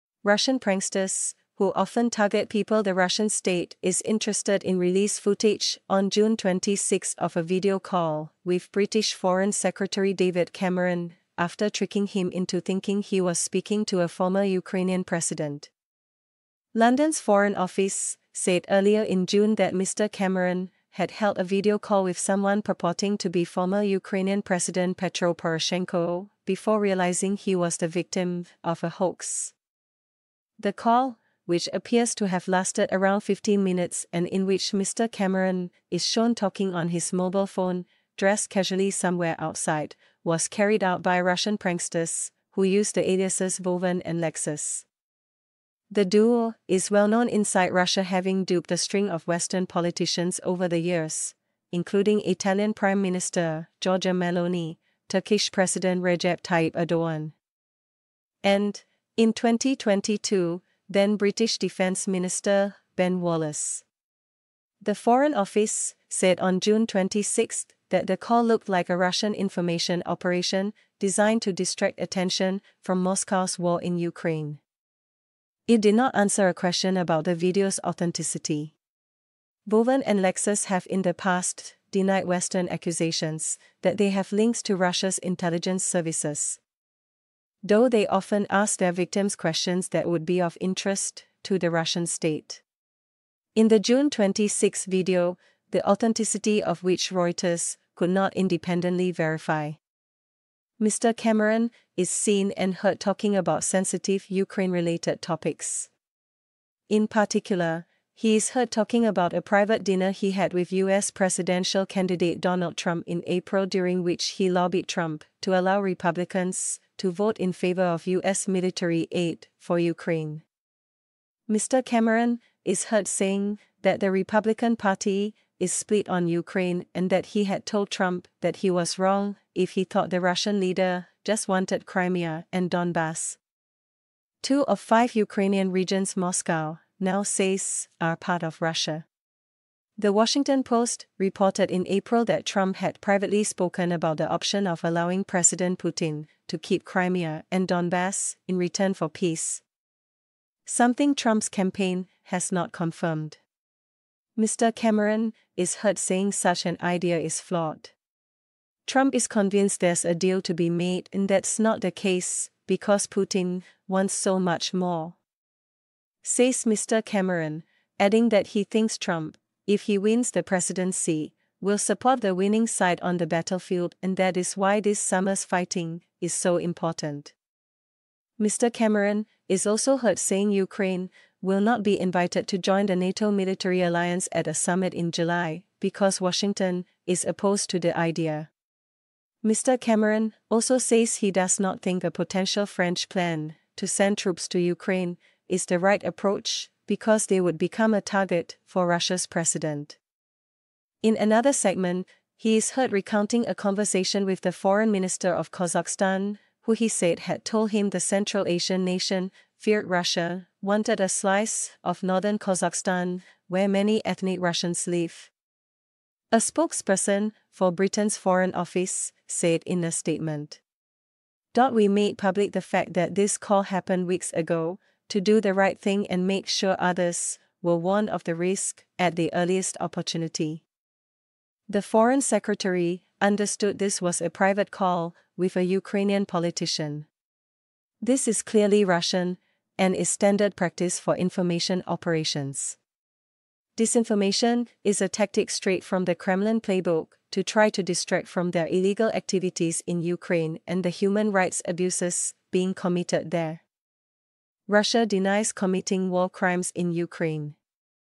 Russian pranksters release hoax video call with Britain’s David Cameron about Ukraine
💗📌 Singapore